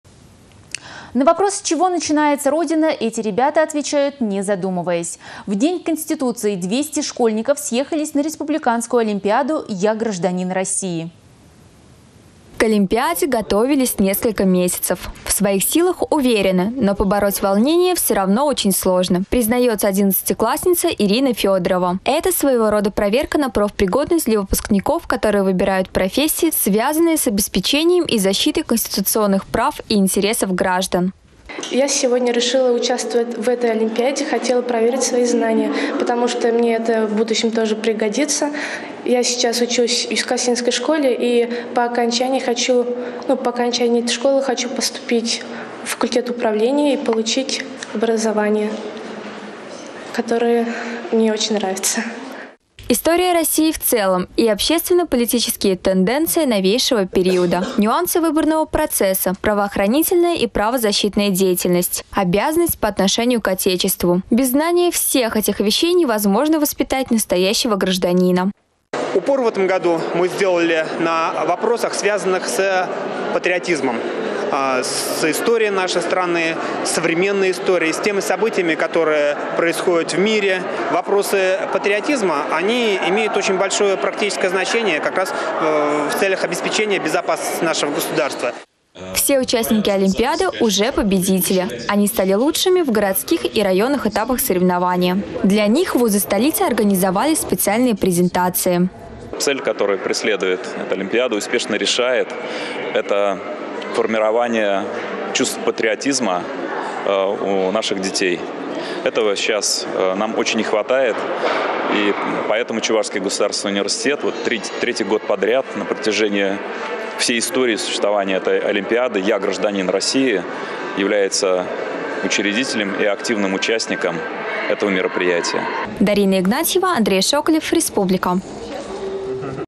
Школьники Чувашии приняли участие в республиканской Олимпиаде «Я — гражданин России»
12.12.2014
 На вопрос «с чего начинается Родина», эти ребята отвечают, не задумываясь. В День конституции 200 школьников съехались на республиканскую Олимпиаду «Я — гражданин России».